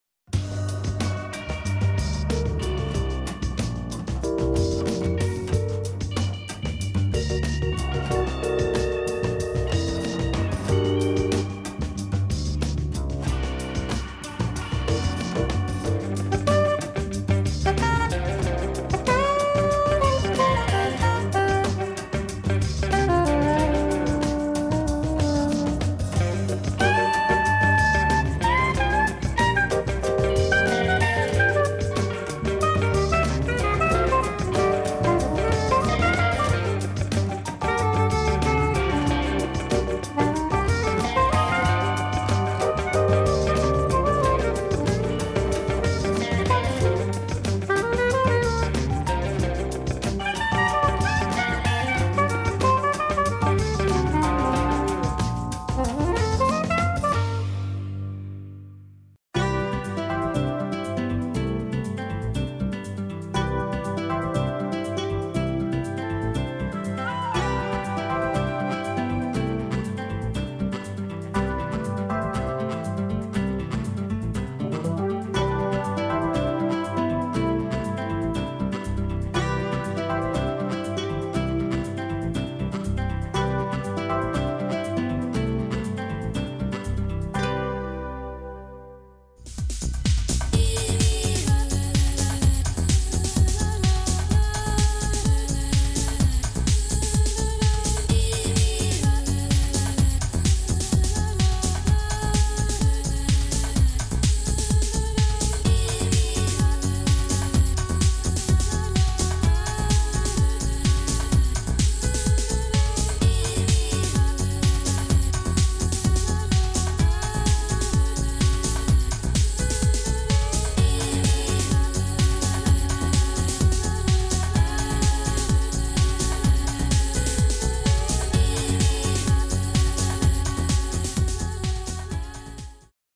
tutto il glamour della Beauty Vogue Night, a Bologna la notte bianca della Moda e della Bellezza !
bellissime immagini dalla Beauty Vogue Night di sabato 9 marzo 2013, la notte bianca della Moda e della Bellezza, in occasione di un Cosmoprof Worldwide 2013, di eccezionale successo - video by Telesanterno